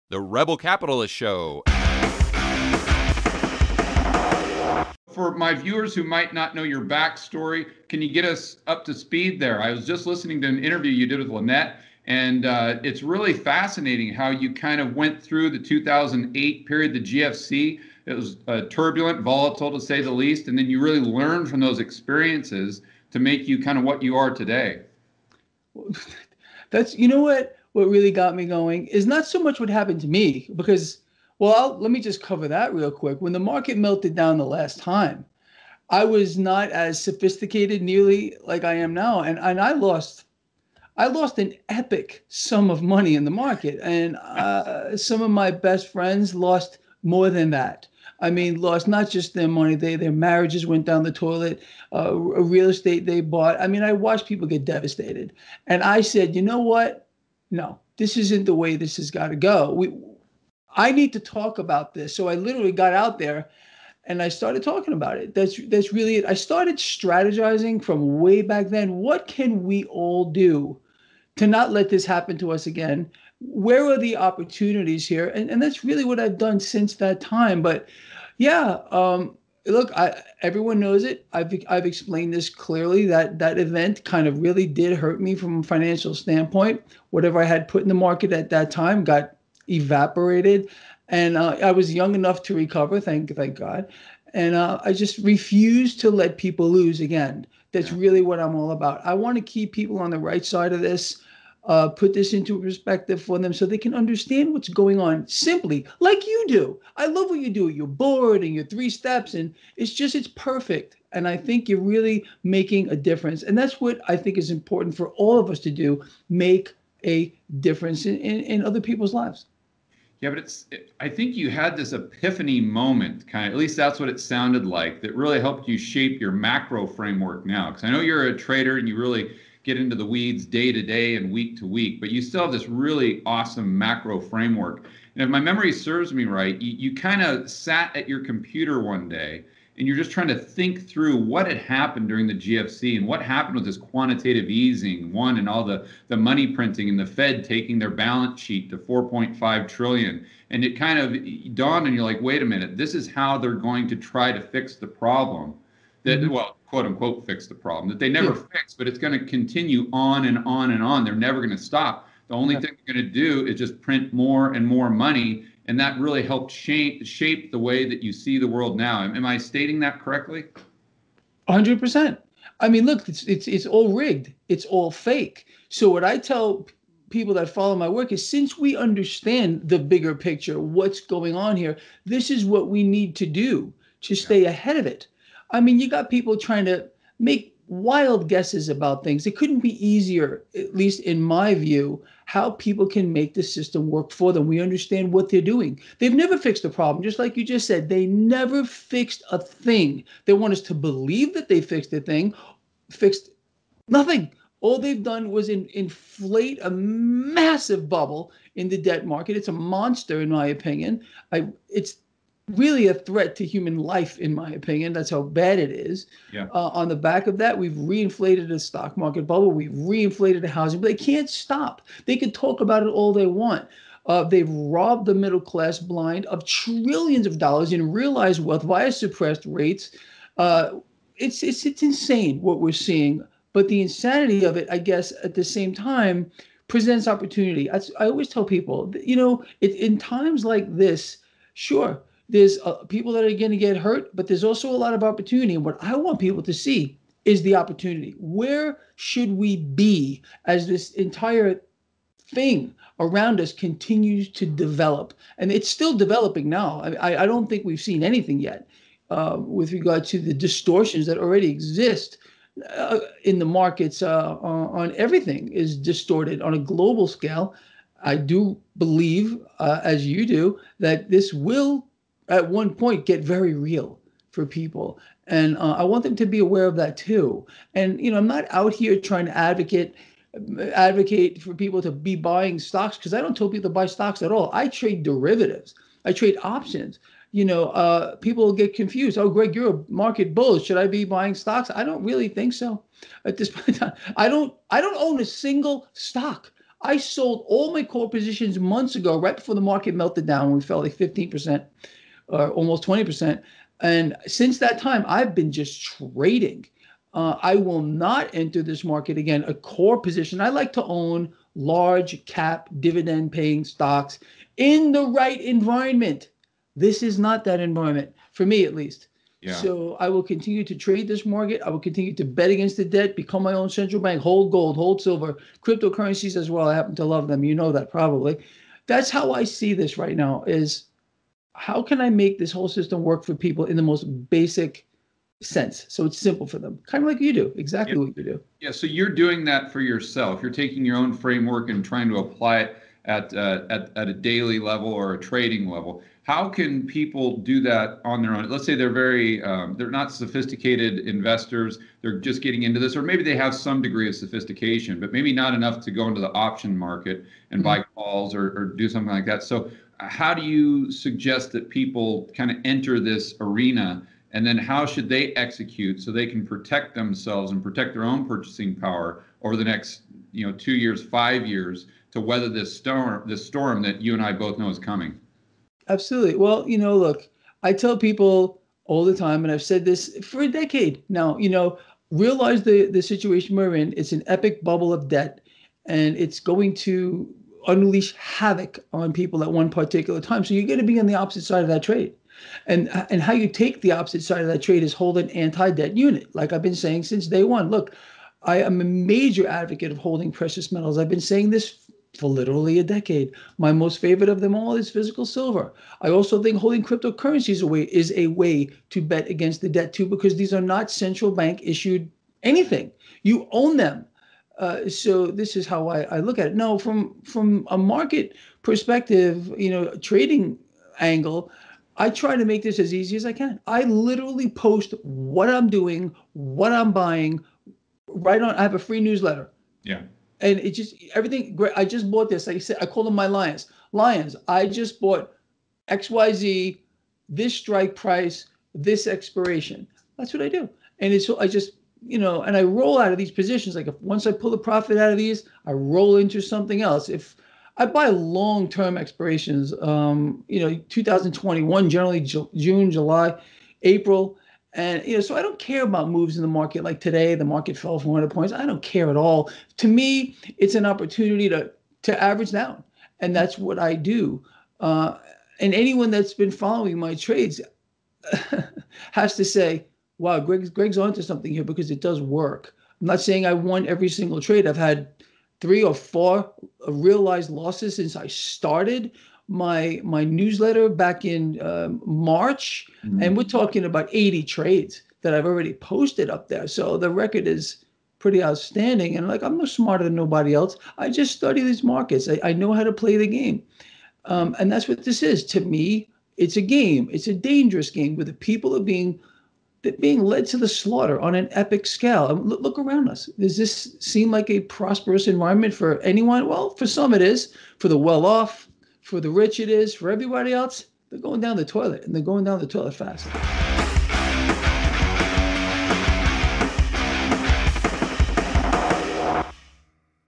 Gregory Mannarino Talks Global Financial Crisis, Learning to Strategize, Trading, Debt (RCS Ep 105)
Interview original date: October 22, 2020

Topics- Gregory Mannarino talks about his experience in times of Global Financial Crisis: his losses, his friends’ losses, and how these events motivated him to strategize and plan for a better investing approach. Trading, macro framework, debt bubble, Stock Market, Housing Market, FED, inflation, trading derivatives, options, choosing the right environment to hold stocks. Bet against debt, hold gold, silver, crypto currencies. How could you apply this framework to your portfolio?